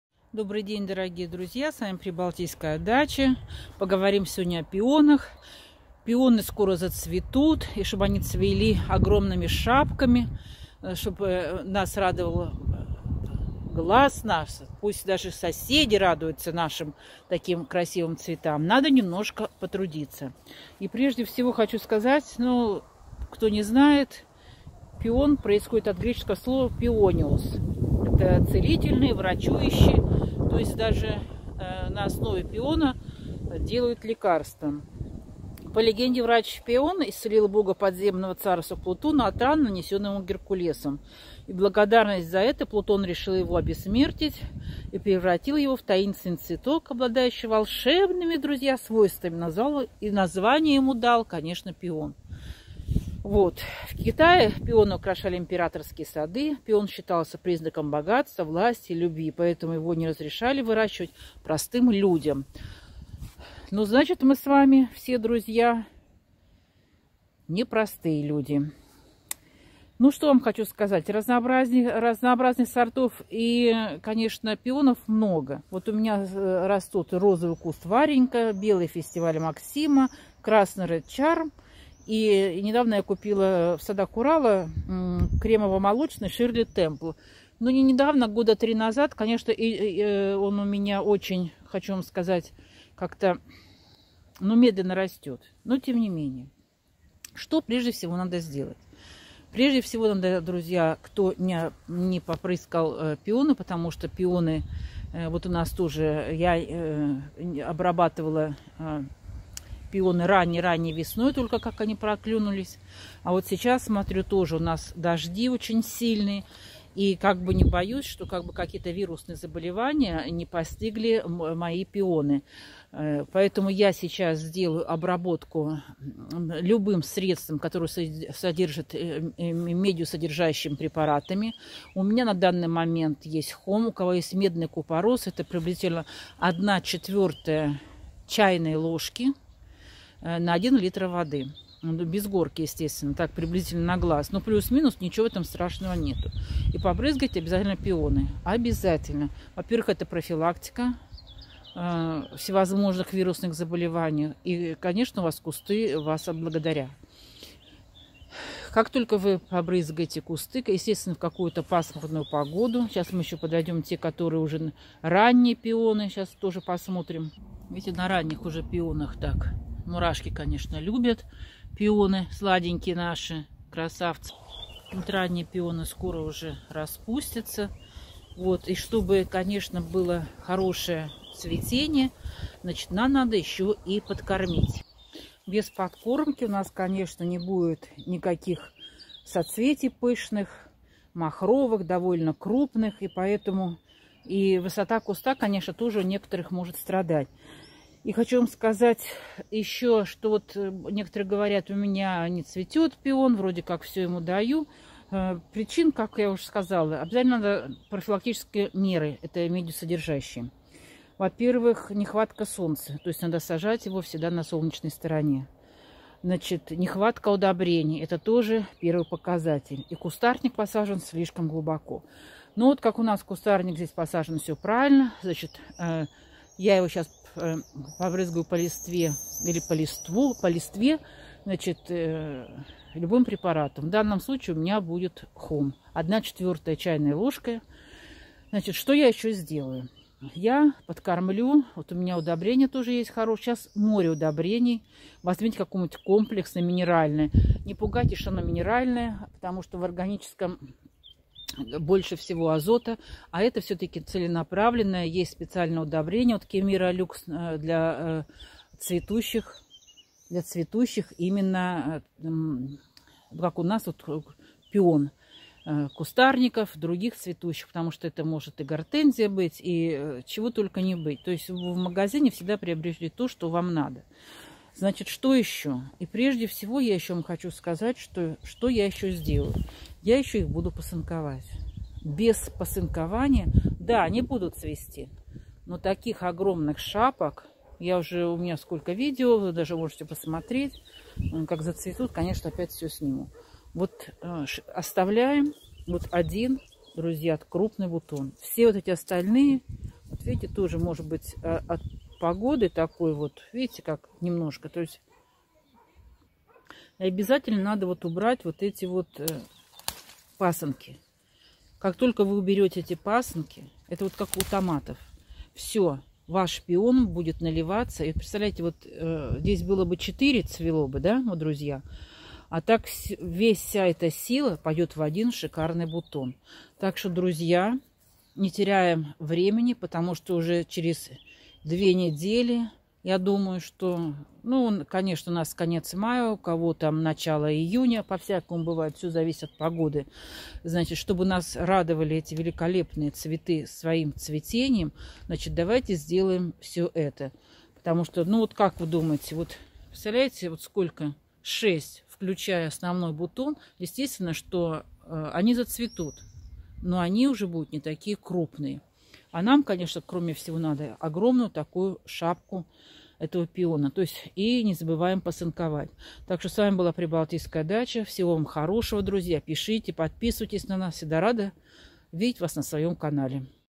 Пионы. Как ухаживать за пионами до цветения.
ПИОНЫ: Пасынкование,  обработка, подкормка. Главный уход за пионами до цветения.